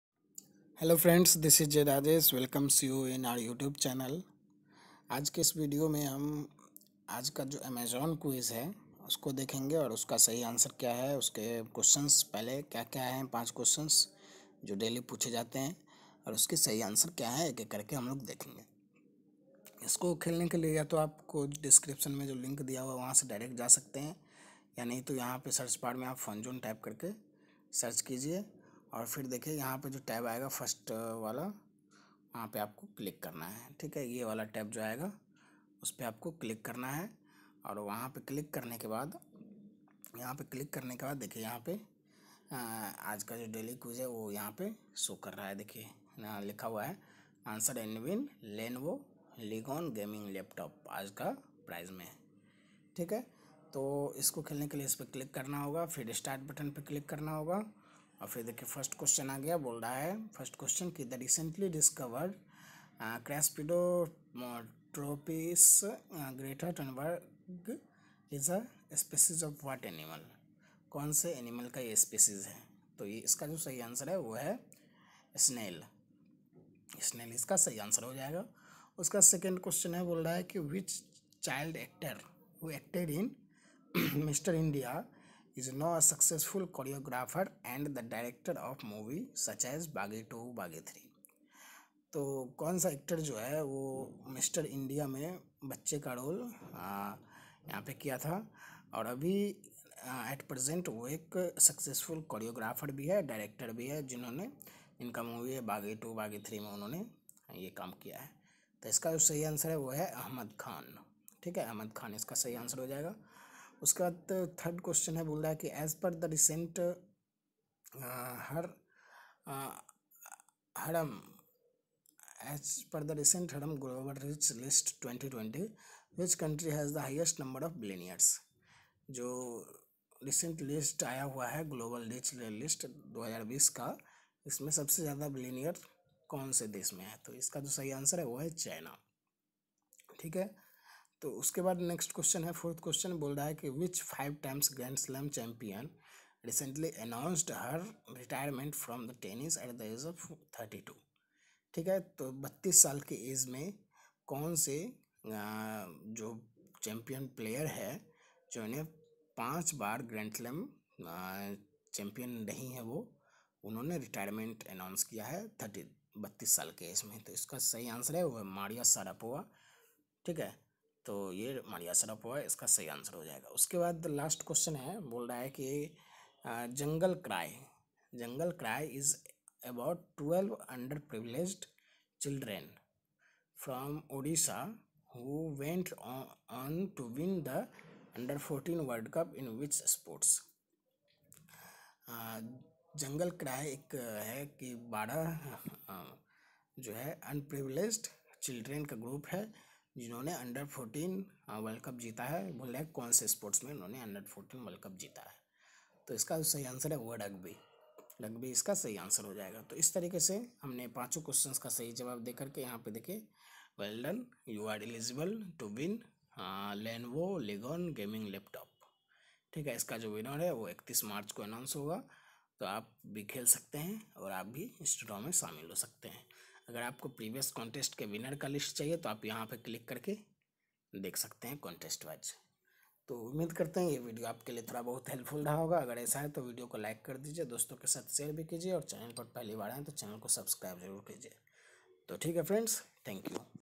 Amazon Daily Quiz Today (14th March 2020) Answer and win a Lenovo Legion Gaming Laptop.
Q1. The recently discovered Craspedotropis gretathunbergae is a species of what animal?

Q2. Which child actor who acted in Mr India is now a successful choreographer and the director of movies such as Baaghi 2 and Baaghi 3?

Q3. As per the recent ‘Hurun Global Rich List 2020’, which country has the highest number of billionaires?

Q4. Which five-time Grand Slam champion recently announced her retirement from tennis at the age of 32?

Q5. ‘Jungle Cry’ is about 12 underprivileged children from Odisha who went on to win the U14 World Cup in which sport?